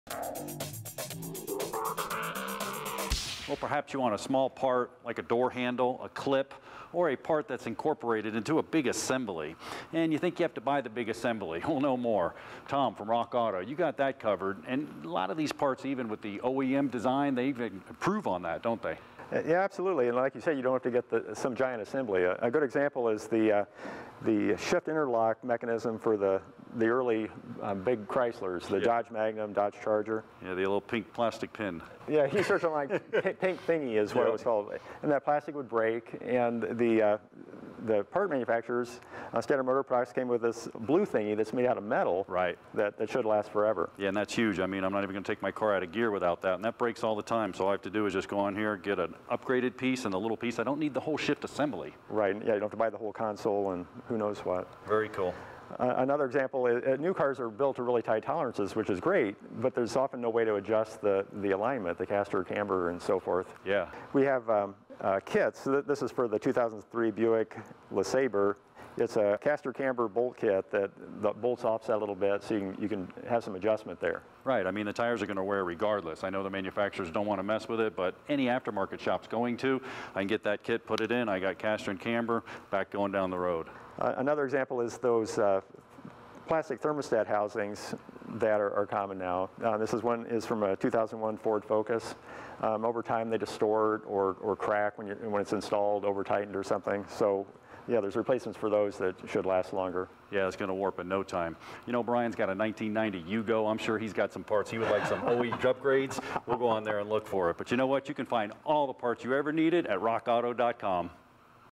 Parts with Improved OEM Design
Replacement parts often correct original equipment parts' problems. John Gardner and Tom Taylor discuss a few examples including Chrysler's "pink thingy," adjustable caster/camber, and plastic thermostat housings.